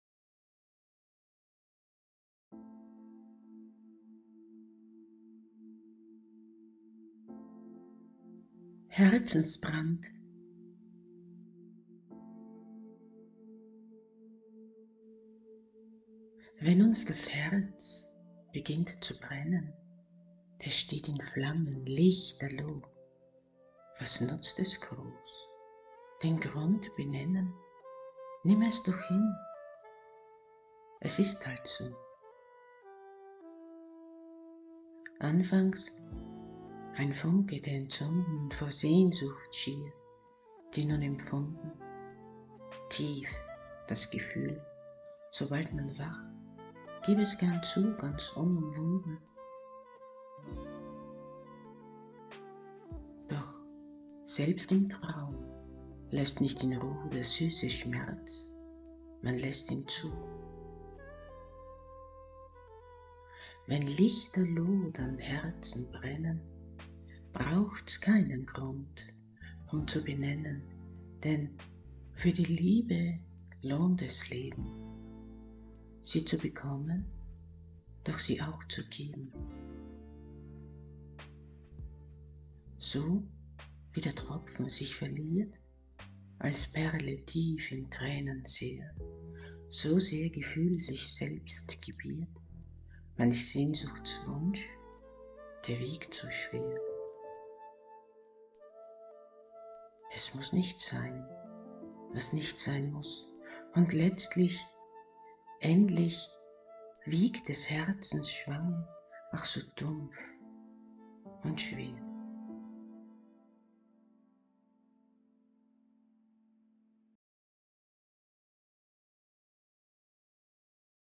Herzensbrand ~ Uschi Rischanek
Wenn uns das Herz
beginnt zu brennen,
es steht in Flammen,
lichterloh.

Was nutzt es groß,
den Grund benennen,
nimm es doch hin,
es ist halt so.

Anfangs ein Funke
der entzunden,
vor Sehnsucht schier,
die nun empfunden,
tief das Gefühl
sobald man wach,
geb es gern zu
ganz unumwunden.

Doch selbst im Traum,
lässt nicht in Ruh',
der süße Schmerz,
man lässt ihn zu.

Wenn lichterloh
dann Herzen brennen,
brauchts keinen Grund
um zu benennen.

Denn für die Liebe
lohnt es leben,
sie zu bekommen,
doch sie auch
zu geben.

So wie der Tropfen sich verliert,
als Perle tief in Tränen sehr,
so sehr Gefühl sich selbst gebiert,
manch Sehnsuchtswunsch
der wiegt so schwer.

Es muss nicht sein,
was nicht sein muss
und letztlich, endlich
wiegt des Herzensschwange,
ach so dumpf und schwer.